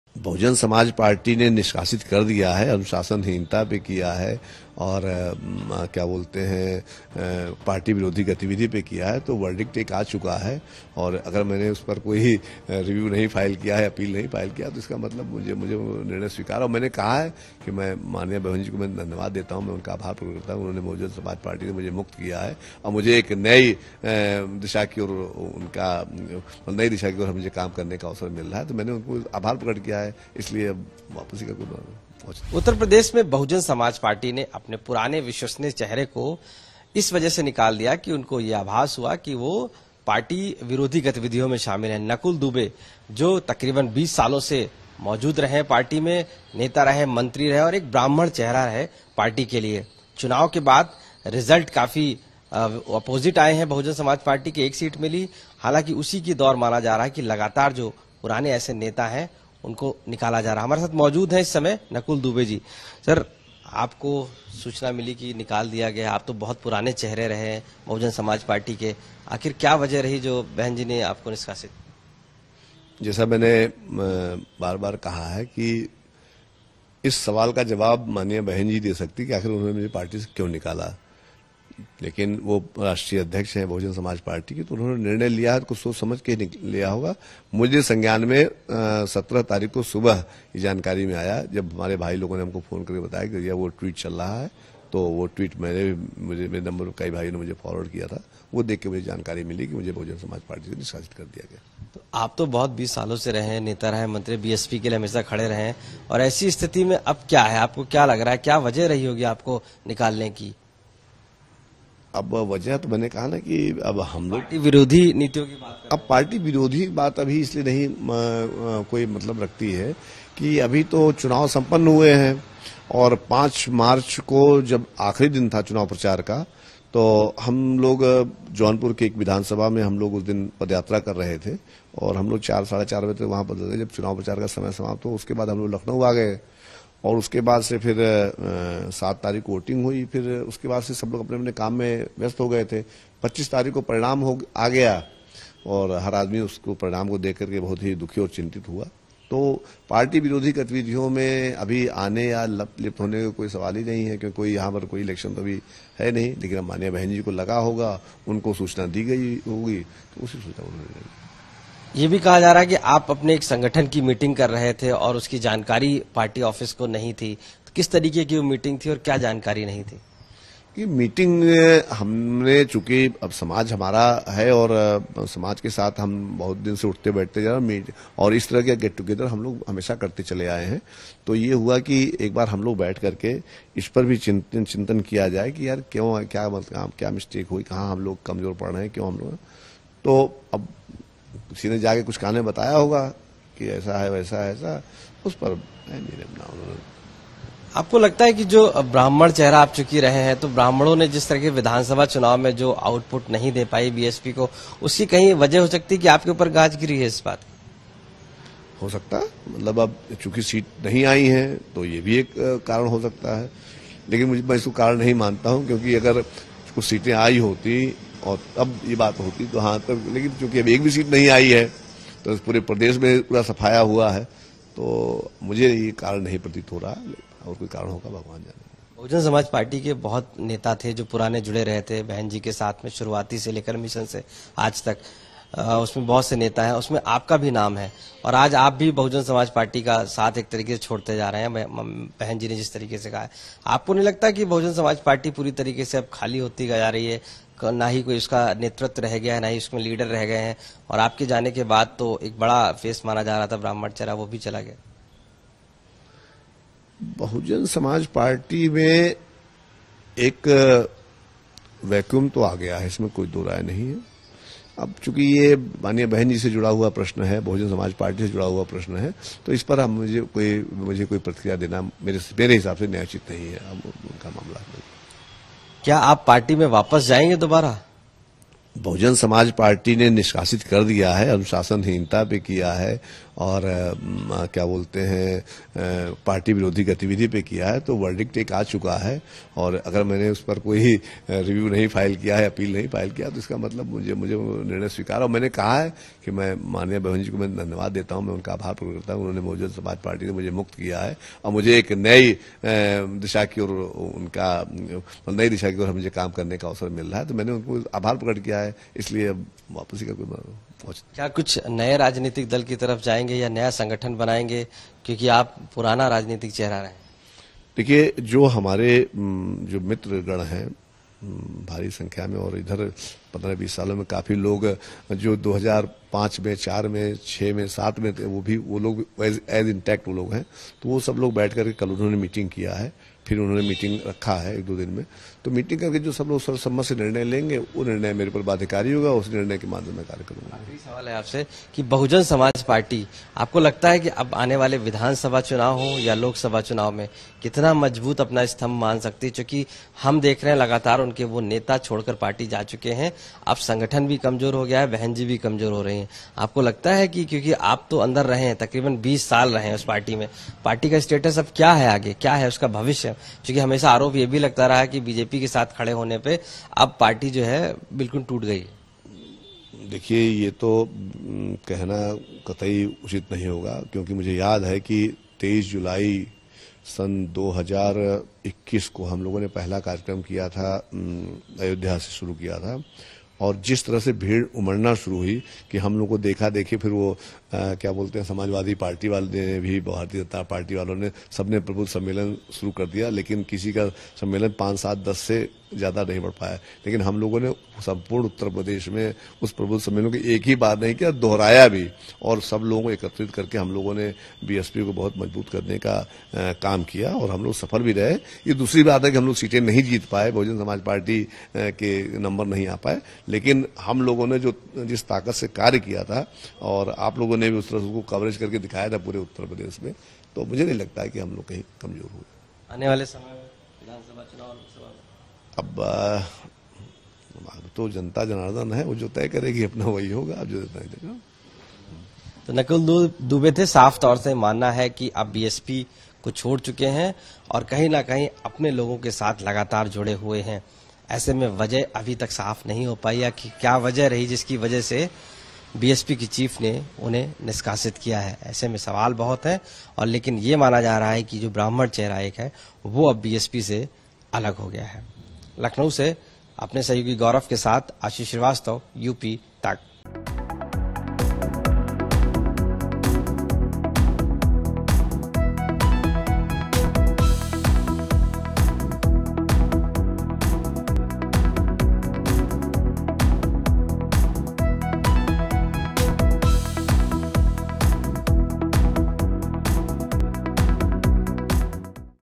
Nakul Dubey : कभी बसपा का बड़ा चेहरा रहे नकुल दुबे को मायवती ने क्यों फेंका पार्टी से बाहर?
बसपा प्रमुख मायावती ने पूर्व मंत्री नकुल दुबे को पार्टी से निष्कासित कर दिया है। इसकी जानकारी उन्होंने ट्वीट कर दी है। जिसमें उन्होंने लिखा है कि पूर्व मंत्री नकुल दुबे को पार्टी में अनुशासनहीनता करने व पार्टी विरोधी गतिविधियों में लिप्त होने के कारण निष्कासित कर दिया गया है। बसपा सरकार व पार्टी में नकुल दुबे की गिनती बड़े ब्राह्मण चेहरे में होती थी..